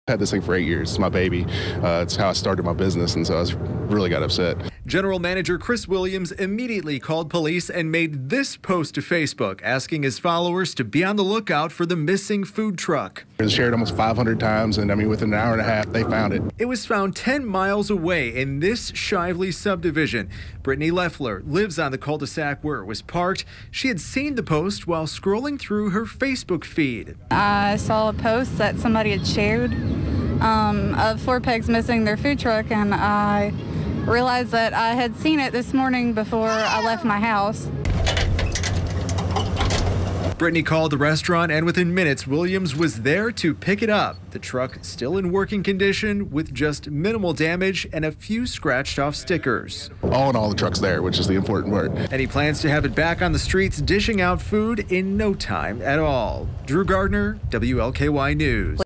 Facebook helps Germantown restaurant track down stolen food truck
The owner of Four Pegs in Germantown has a lot to be thankful for after his food truck was stolen and then found all in one day.